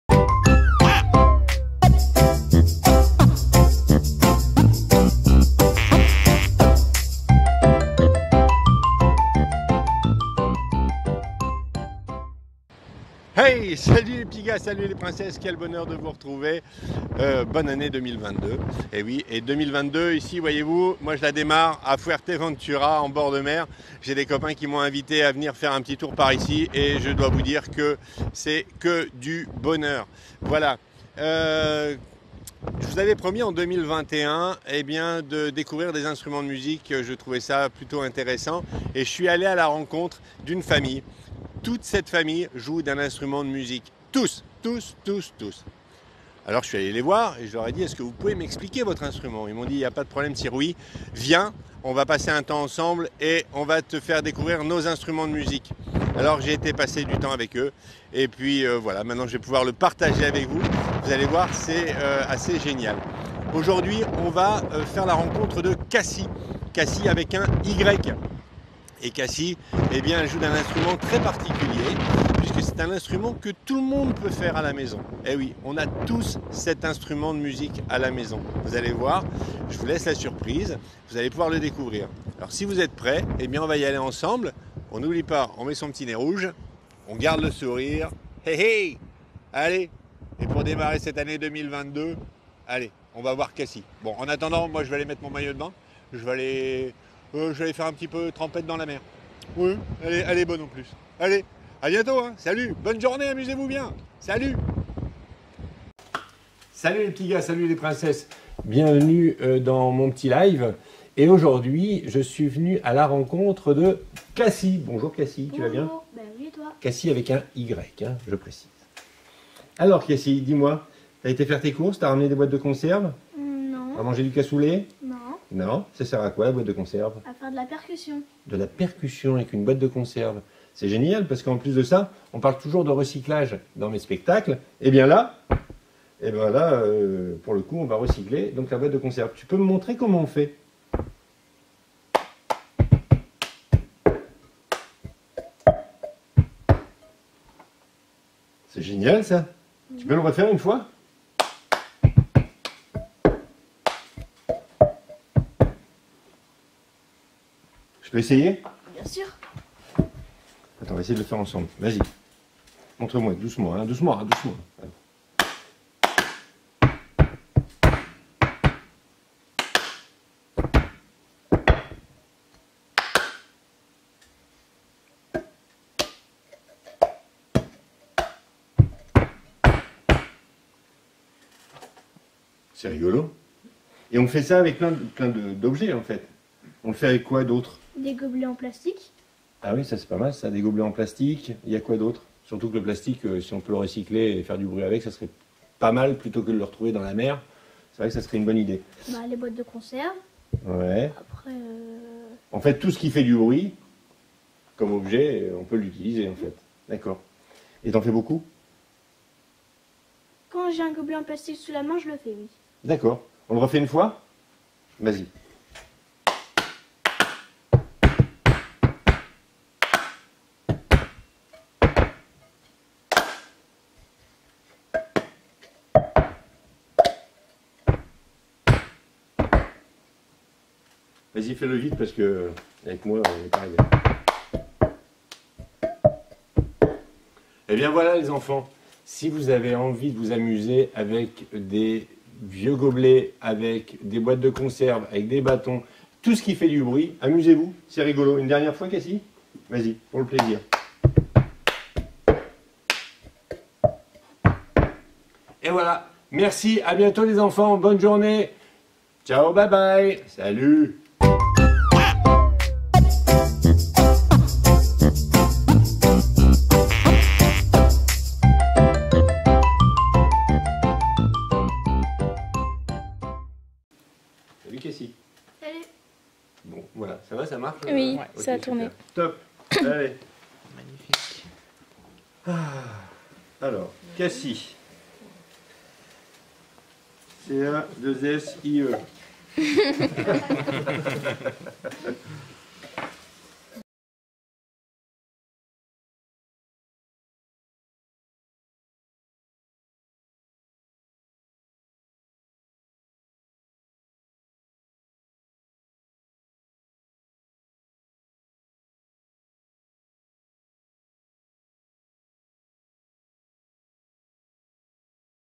Les instruments de musique de Sirouy - découverte d'instruments de musique pour enfants -
Retrouvez les instruments les différents instruments de musique avec Sirouy le clown. Sirouy parcourt la France pour sensibiliser les enfants à la preservation de la planète et la santé. Son Leitmotiv, apprendre en s'amusant, chantant, dansant !
Vous pouvez retrouver Sirouy dans les écoles maternelles, écoles primaires, centres sociaux, comités d'entreprises (C.E).